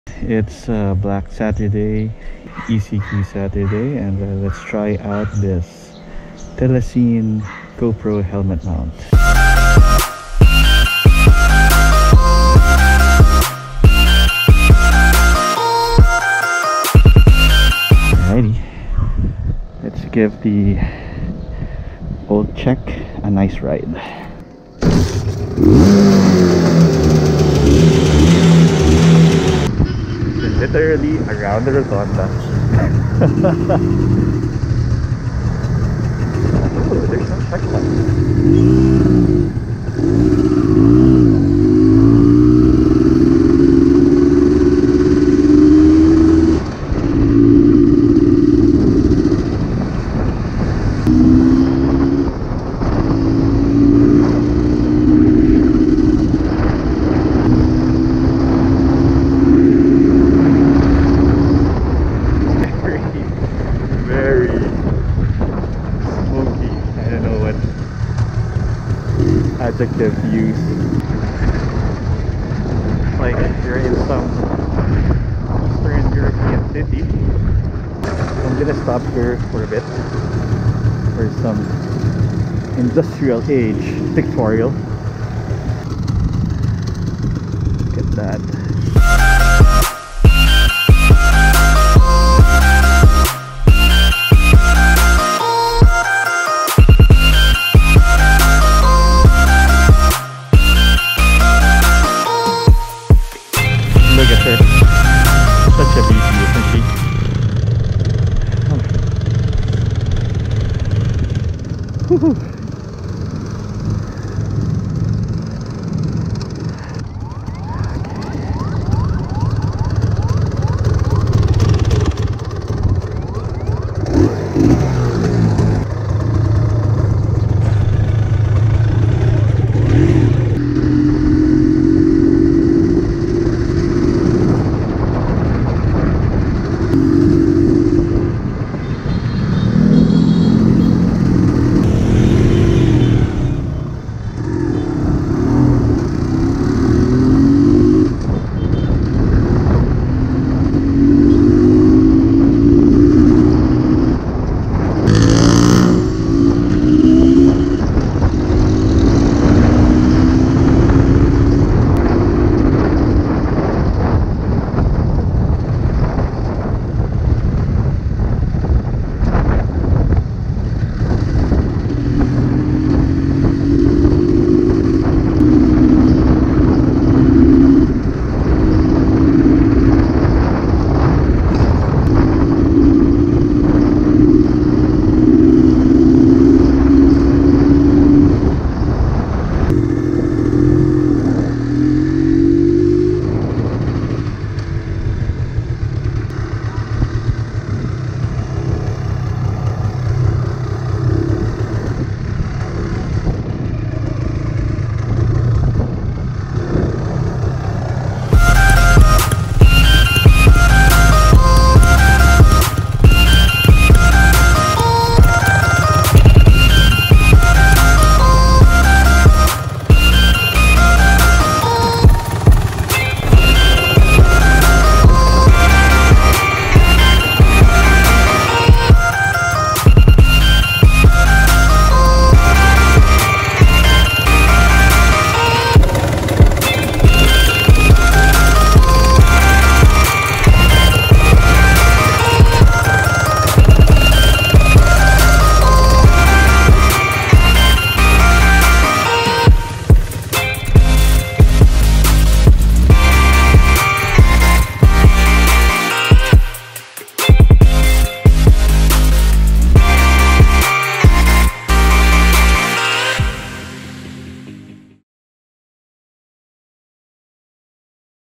Jawa Type 593 + Telesin action camera helmet chin mount
Went out for a short spin on the Jawa Type 593 with a GoPro Hero 6 attached to a Telesin helmet chin mount. There's more to the story but will upload this short version first. Cheerio!